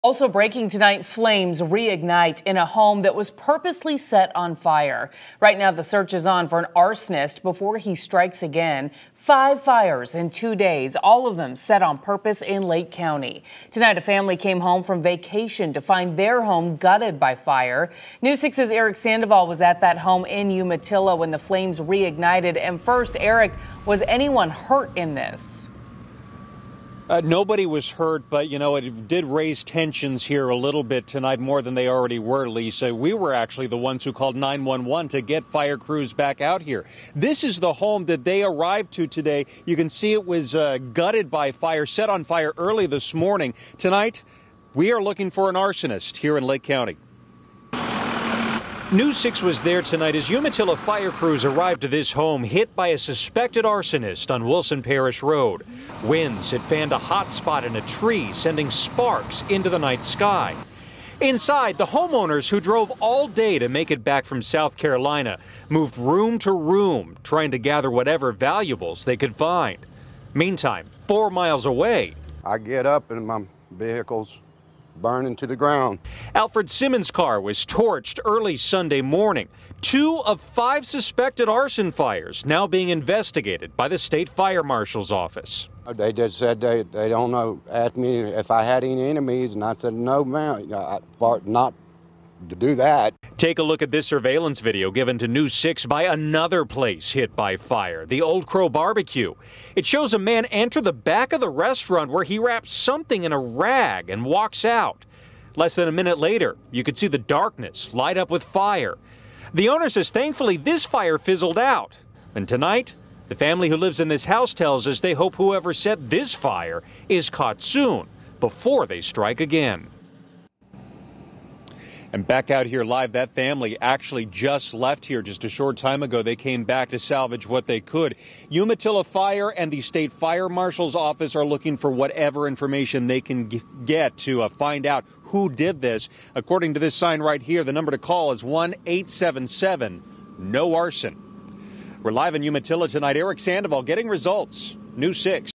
Fire reignites after string of arsons
A fire reignited after a string of arsons in the Umatilla area of Lake County.